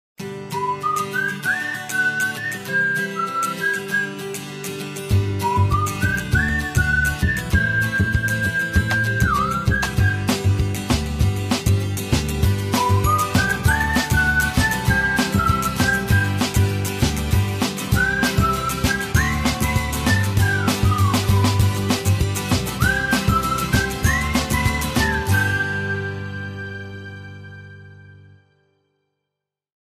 Bayeta microfibras y paño especial para vajillas
La bayeta de microfibras es uno de los productos de limpieza más polivalentes comercializados por Clim Profesional, ya que puede utilizarse en todo tipo de trabajos relacionados con la limpieza profesional, como la hostelería, la restauración, la industria alimentaria o en hospitales. Por su parte, el paño especial para vajillas es ideal para el secado de cristalería, cubertería y artículos de cocina.